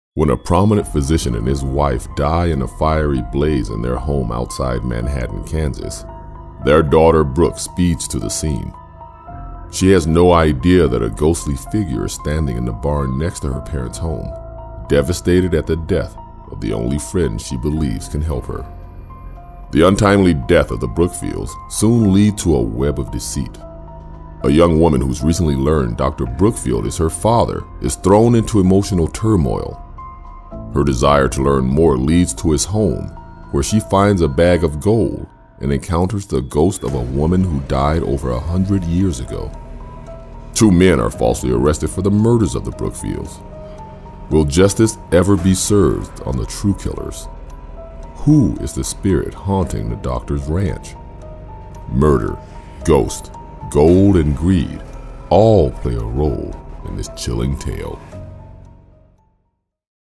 Sparks Fly at Midnight by Christin Hepner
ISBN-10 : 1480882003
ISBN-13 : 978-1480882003

When a prominent physician and his wife die in a fiery blaze in their ranch home outside Manhattan, Kansas, their daughter, Brooke, speeds to the scene. She has no idea that a ghostly figure is standing in the barn next to her parents' home, devastated at the death of the only friend she believes can help her.

The untimely deaths of the Brookfields soon lead to a web of deceit. Nicole Martin, who moved to Manhattan to attend college, has recently learned Dr. Brookfield is her father. After his sudden death throws her into an emotional tailspin, her desire to see his home leads her to his ranch, where she finds a bag of gold and encounters a ghost who lived more than a century past. Meanwhile, two men recently released from prison are falsely arrested for the murders of the Brookfields. Will justice ever be served to the true killers? And who is the ghost haunting the doctor's ranch?

In this chilling tale, murder, a ghost, gold, and greed all play a role in a complex mystery after a physician and his wife die in a fiery fire.